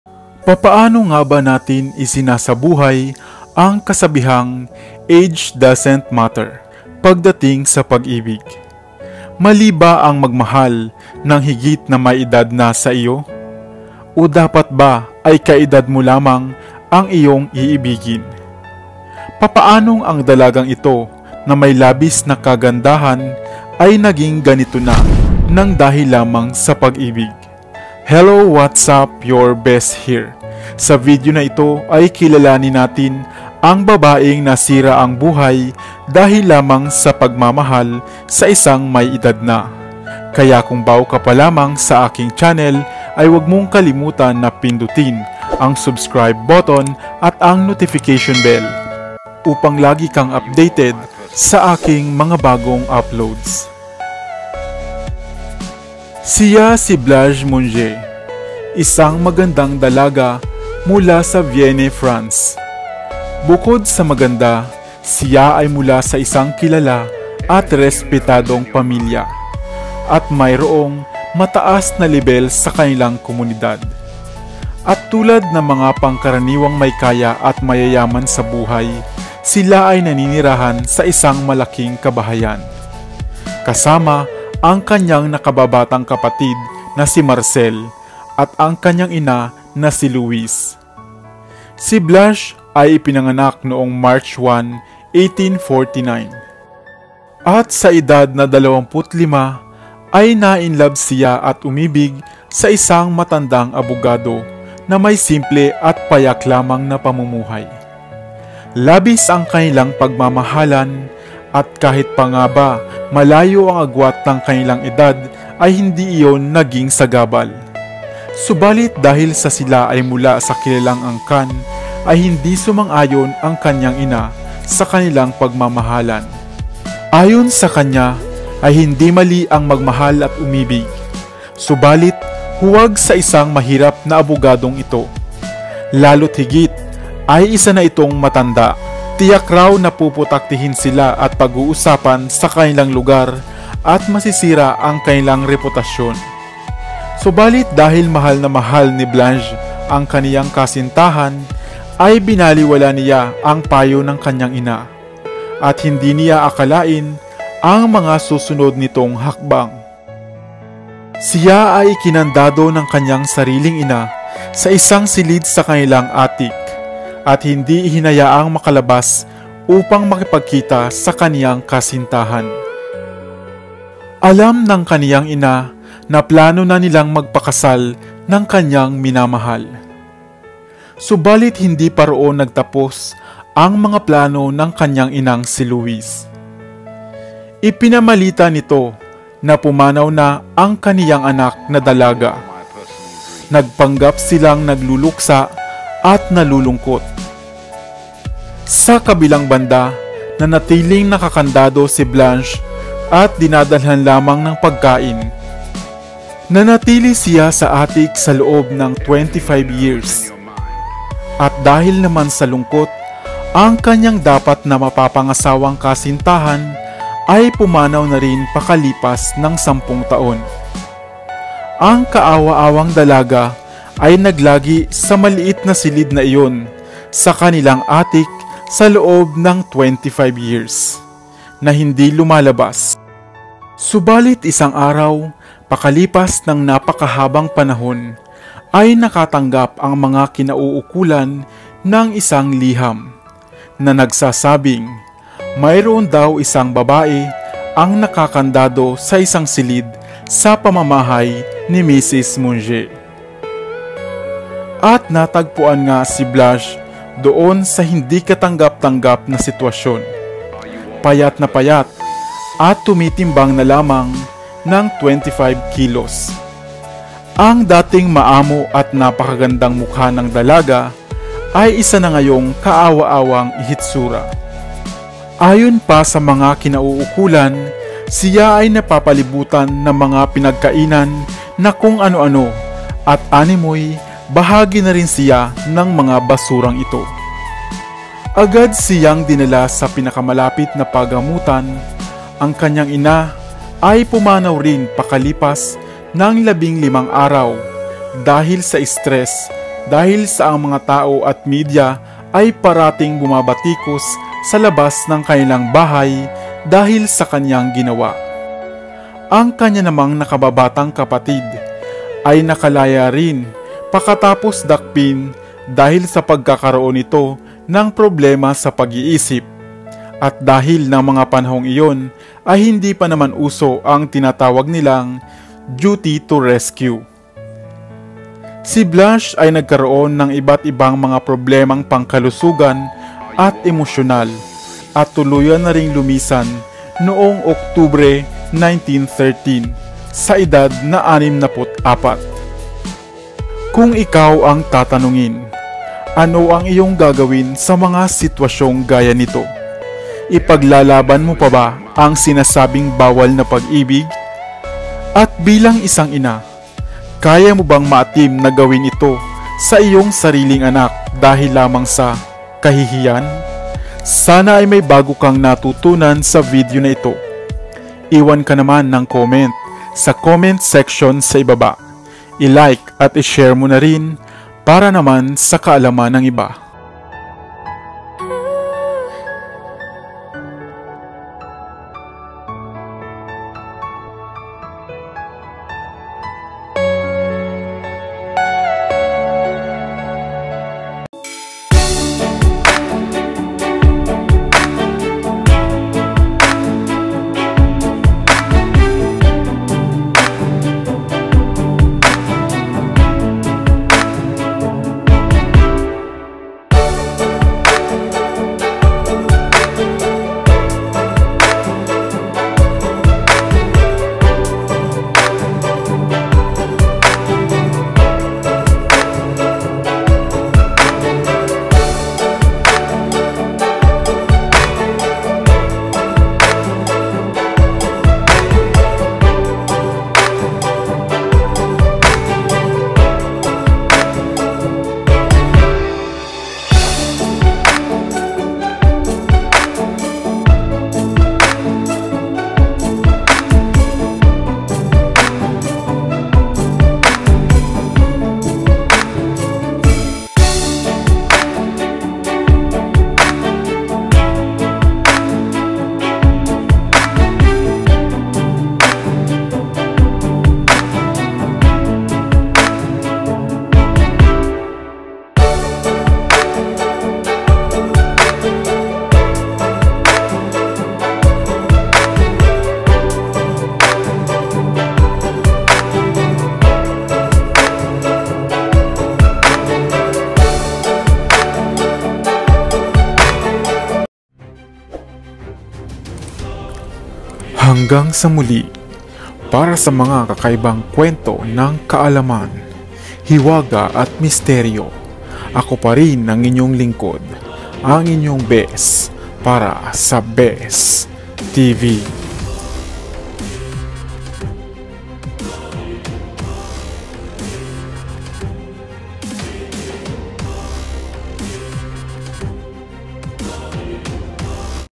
BABAENG NAGMAHAL NG ISANG MATANDANG NOBYO , GANITO ANG KANYANG SINAPIT | True Story | Bhes Tv
Isang babae ang nagmahal ng isang matandang nobyo. at dahil sa desisyon niyang ito hindi niya akalain na hahantong pala sila sa ganitong sitwasyon at kalagayan .Panoorin ang nakakahabag na kwento ng isang dalaga na. Nagmahal lamang ng matanda ay nagka ganito na ...